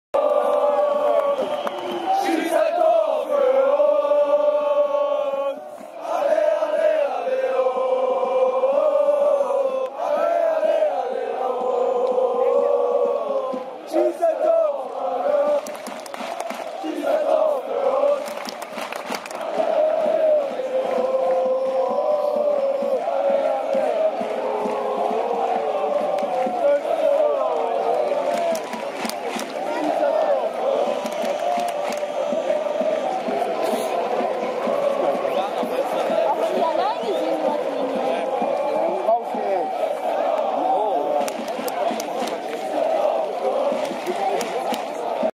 Rot Weiß Oberhausen - Fortuna Düsseldorf - ULTRA Stimmung Block 42 - 02.05.2010 RWO F95
ULTRA Stimmung Block 42  - SC Rot Weiß Oberhausen - Fortuna Düsseldorf - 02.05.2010 - RWO  F95 - 2. Liga das Spiel endete 0:1